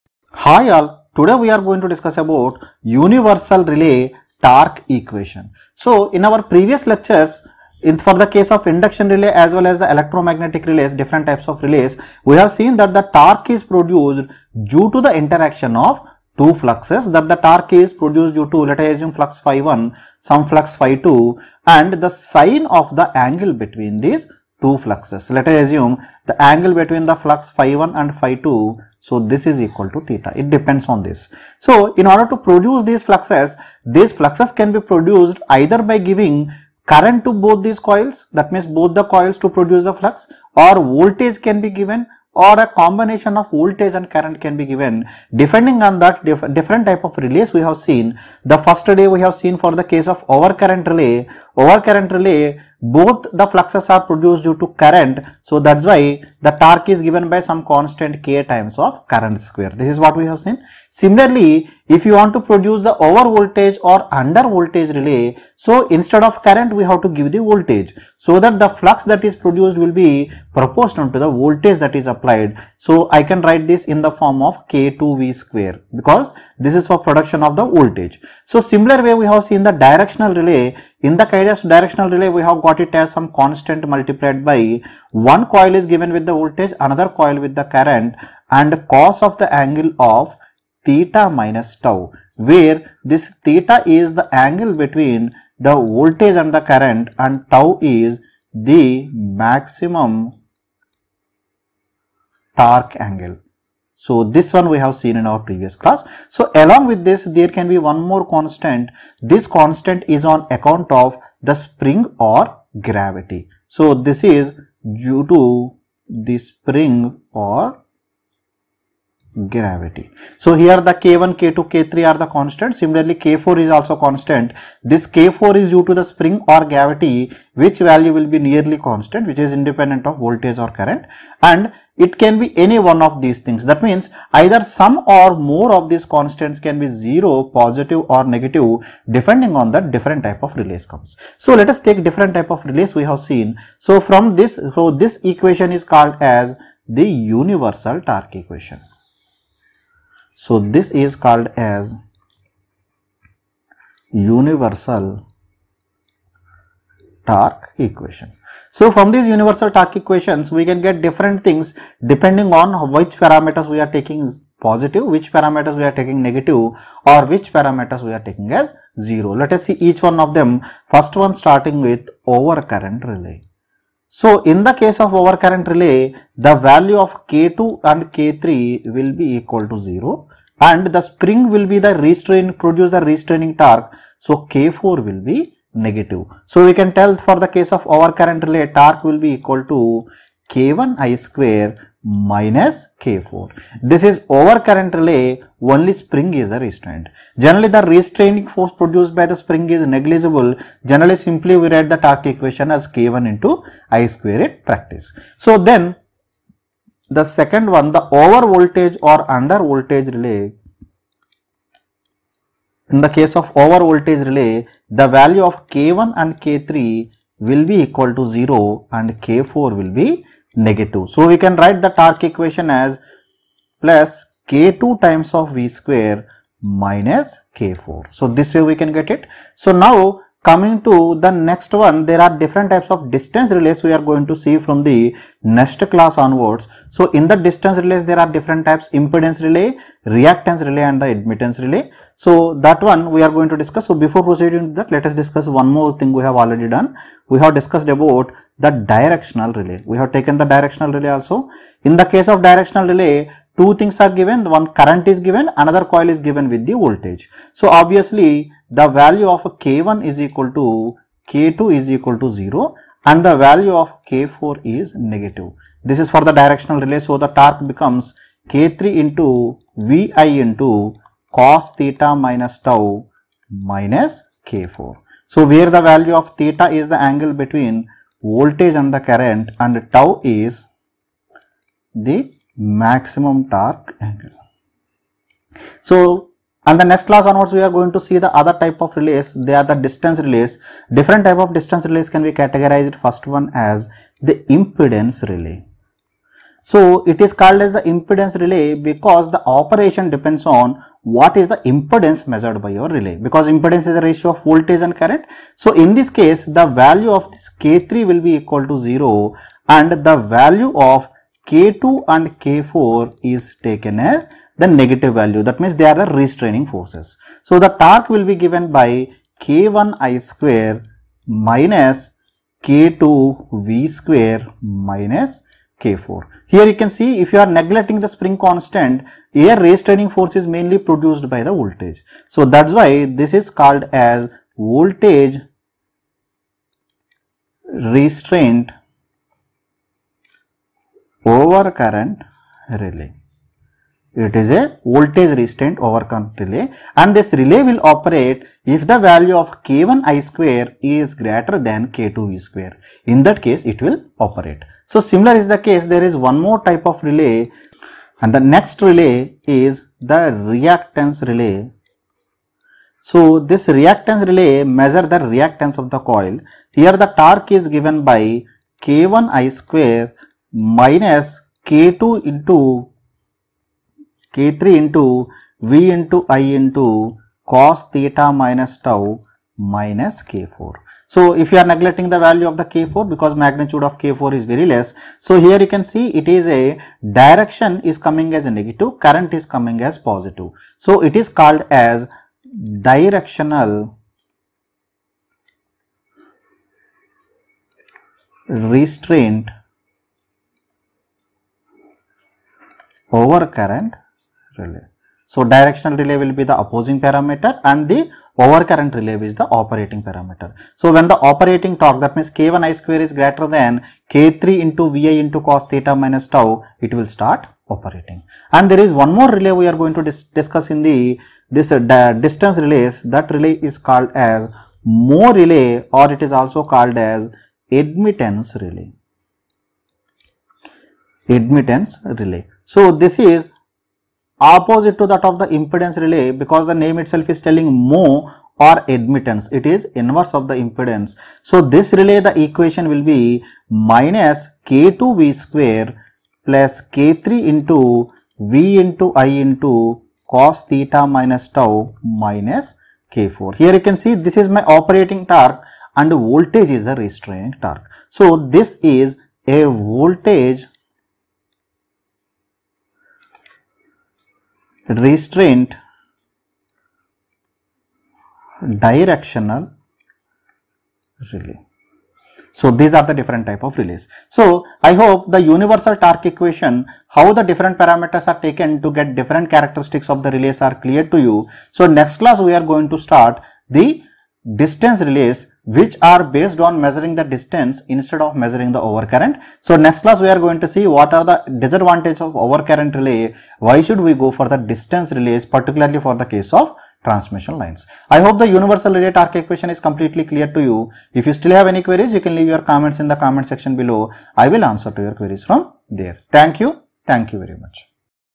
SGP313 Universal Relay Torque Equation
Lectures on Switchgear & Protection By Dr. Tirupathiraju Kanumuri, Assistant Professor, NIT Delhi